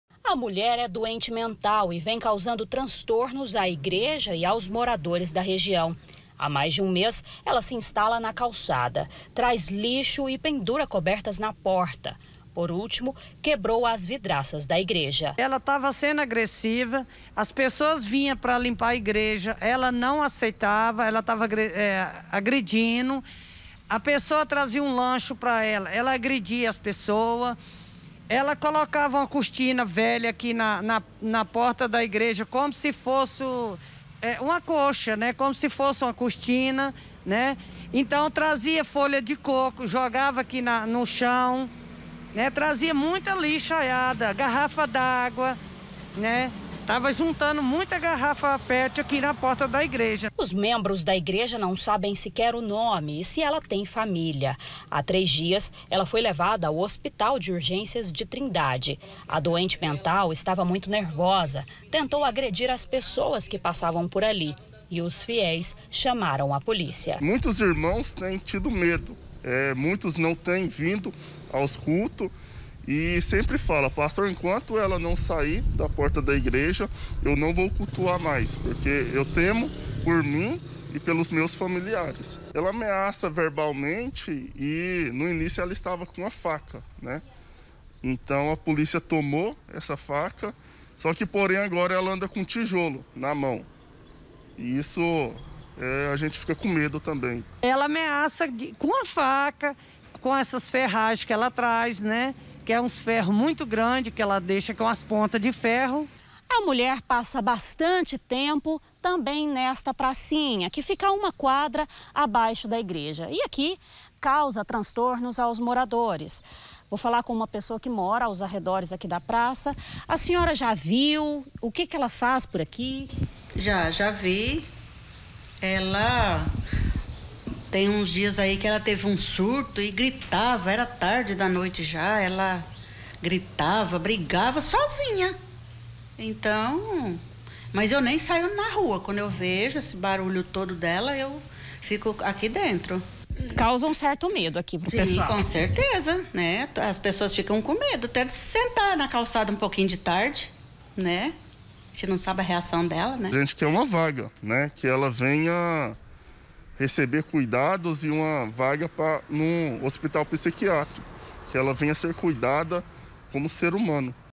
VIZINHOS ASSUSTADOS MORADORA DE RUA AGRESSIVA AMEAÇA AS PESSOAS
UMA MORADORA DE RUA BASTANTE AGRESSIVA ESTÁ AMEAÇANDO MORADORES E FREQUENTADORES DE UMA IGREJA EM TRINDADE. ELA JÁ CHEGOU A USAR UMA FACA PARA INTIMIDAR AS PESSOAS. ELES QUEREM QUE ELA SEJA INTERNADA NUMA CLÍNICA PSIQUIÁTRICA.